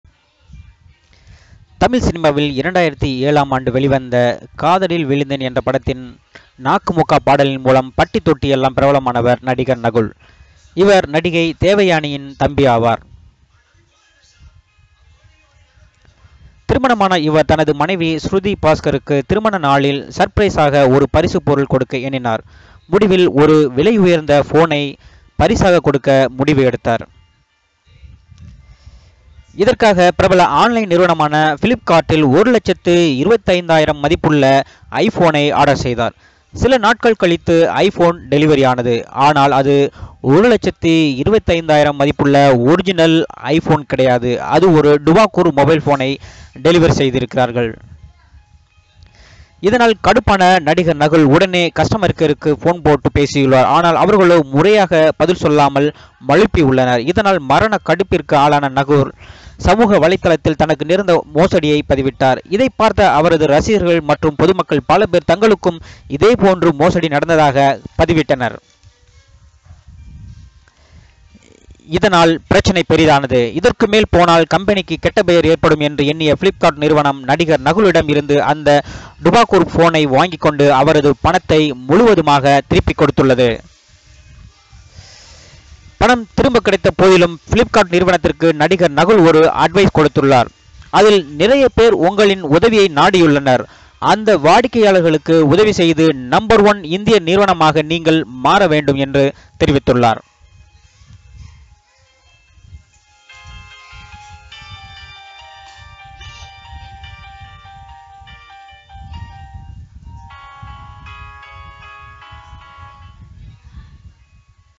நடிகர் நகுலிடம் மோசடி செய்த பிரபல ஆன்லைன் நிறுவனம்
Please watch this video which is about "ONLINE FRAUD".